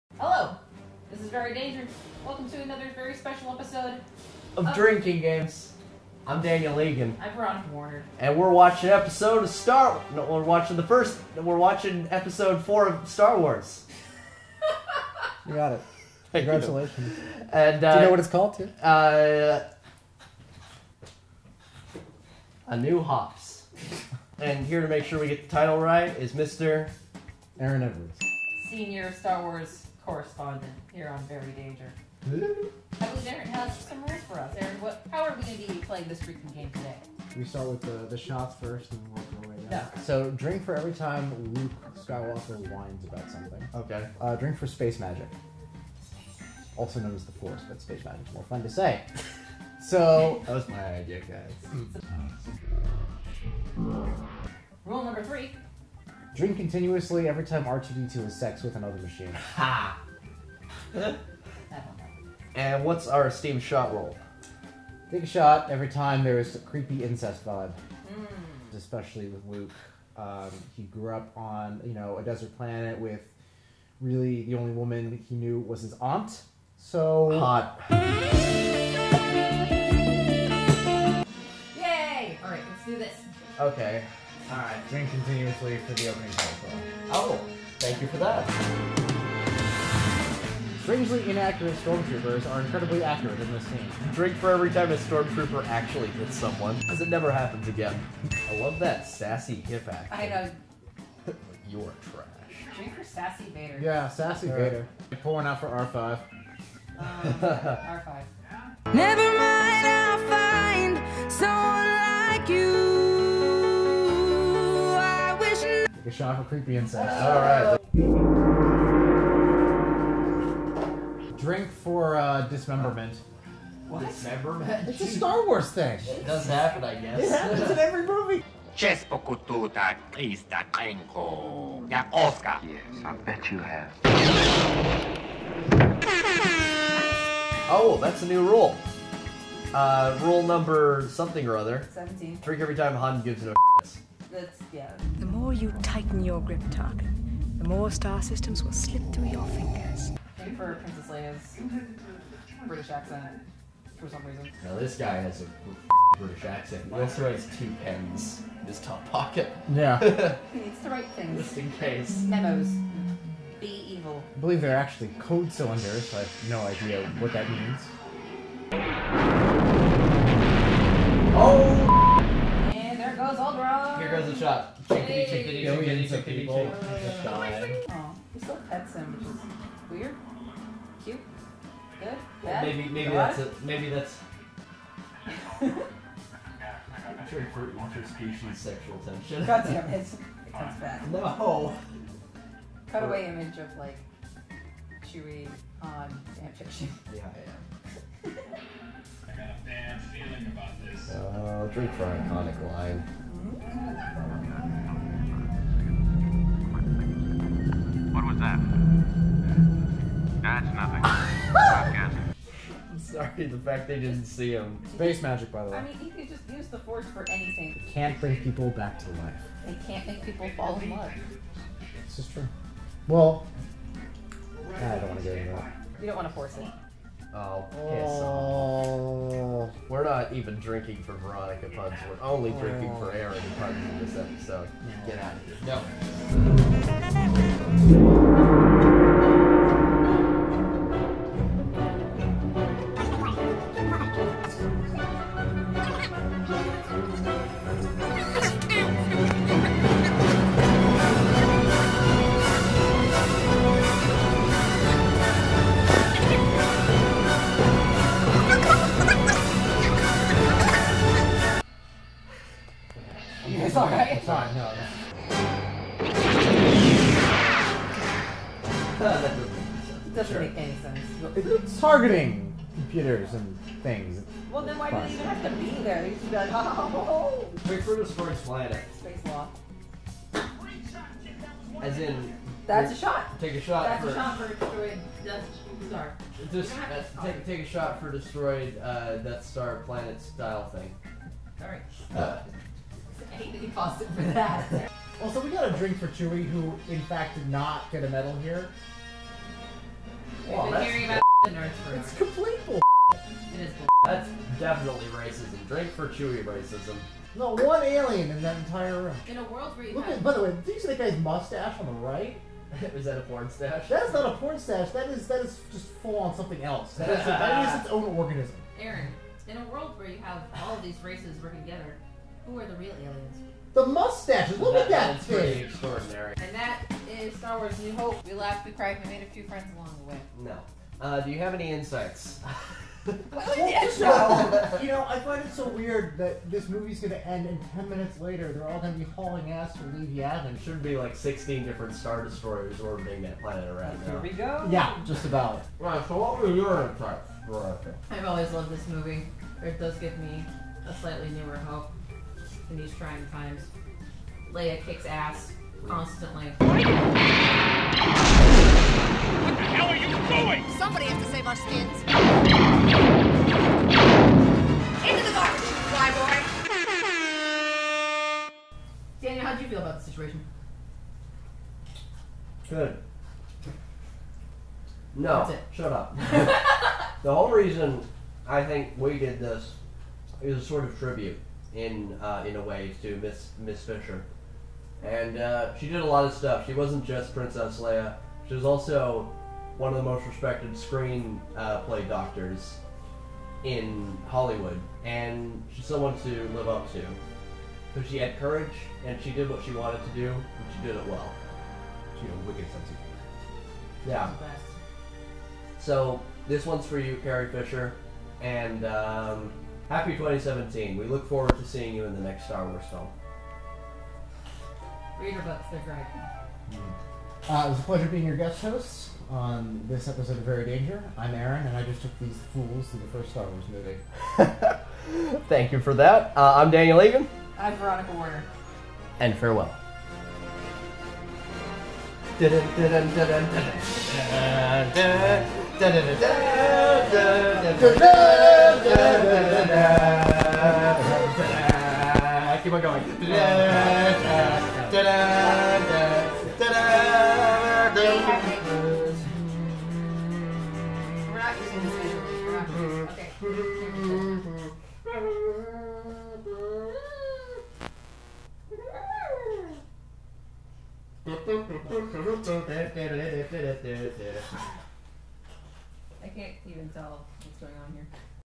Star Wars | Drinking Games
Very Danger makes the Kessel Run in less than twelve parsecs, with guest star and Star Wars brainiac, Aaron E! Join us as we embark on an epic mission to drink more alcohol than our nervous systems can manage! Spoiler Alert: it's a lot. May the force be with you!

THE SOCIALS